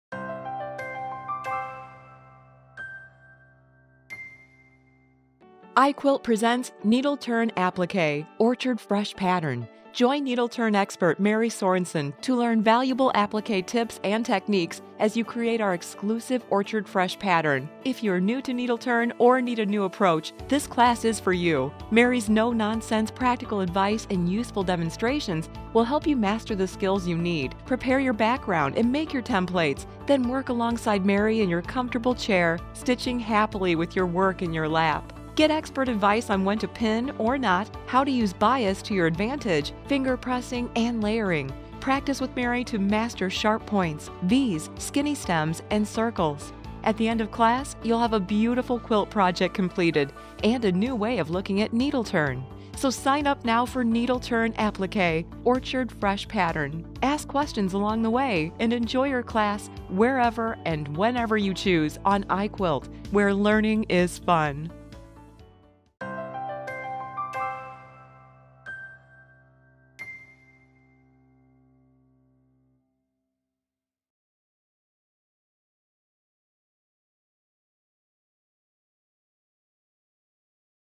Needleturn Appliqué: Orchard Fresh Pattern iquilt Course
Learn valuable needleturn appliqué tips and techniques as you make the unique Orchard Fresh pattern alongside expert appliqué artist Mary Sorensen. Whether you are new to needleturn or simply need a new approach, Mary'a iquilt.com class is for you.